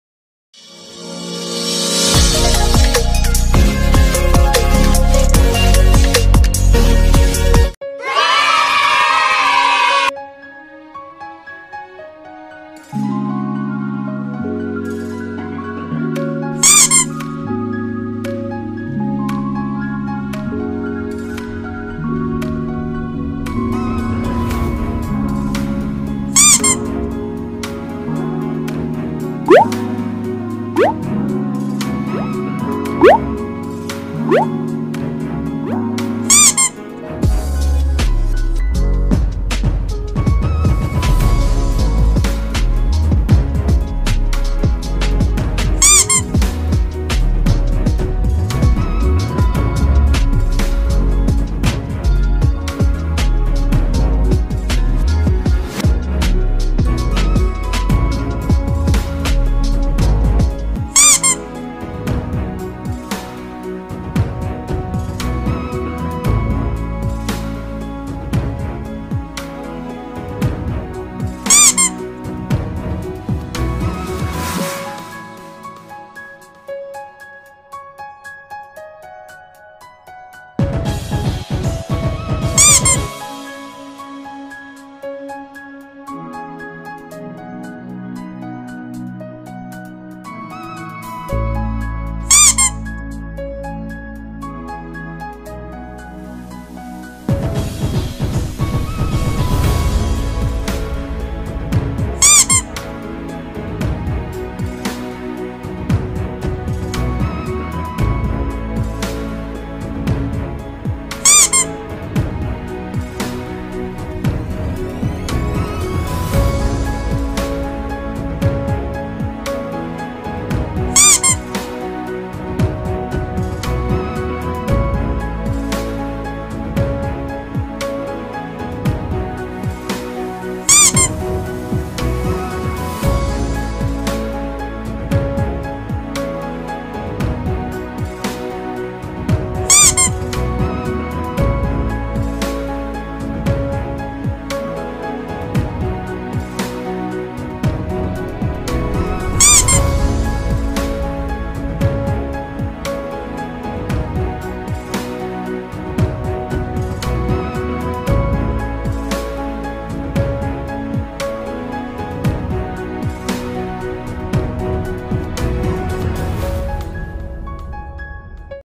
How to draw baby pacifier step by step || Soni Drawing Art
About this video :- Hello Guys,. This is Abhishek from Hazaribagh. Today I draw a Pacifier in easy way . So enjoy it at your own risks 😄😄